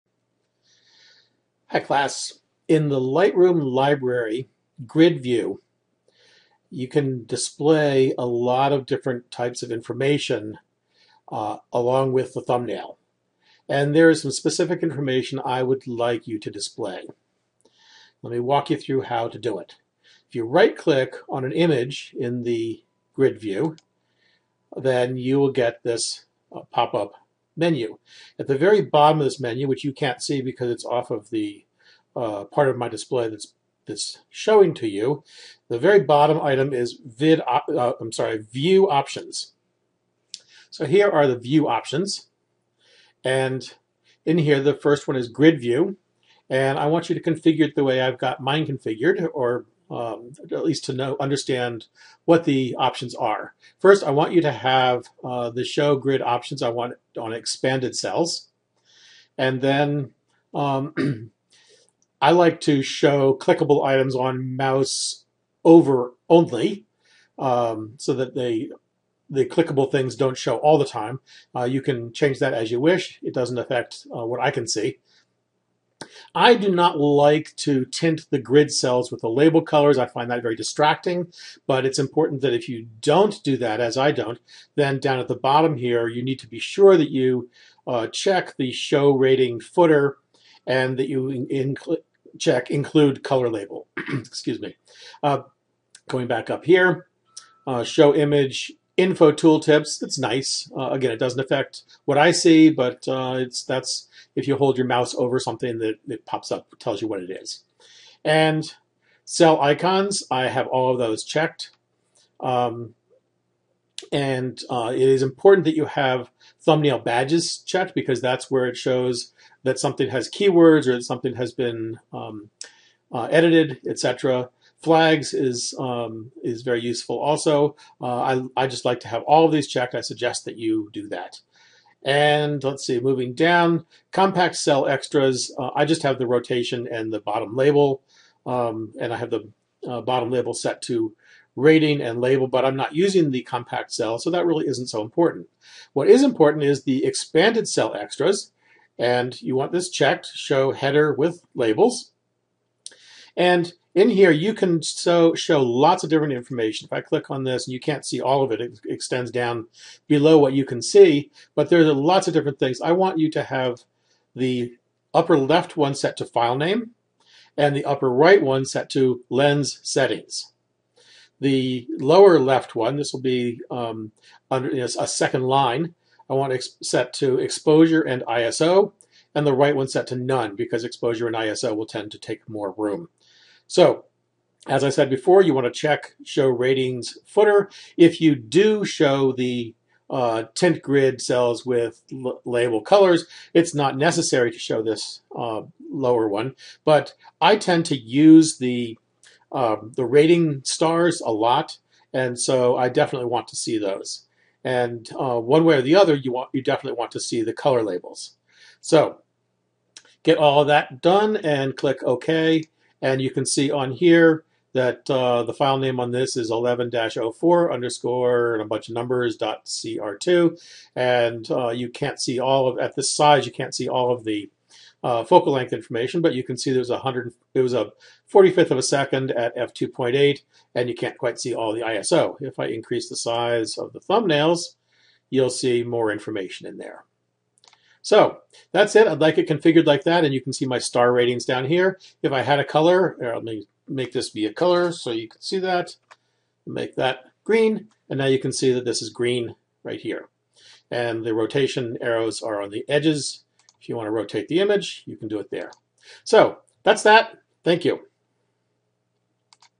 Lightroom Thumbnail Info
How to display file and exposure data with Lightroom thumbnails in the Grid view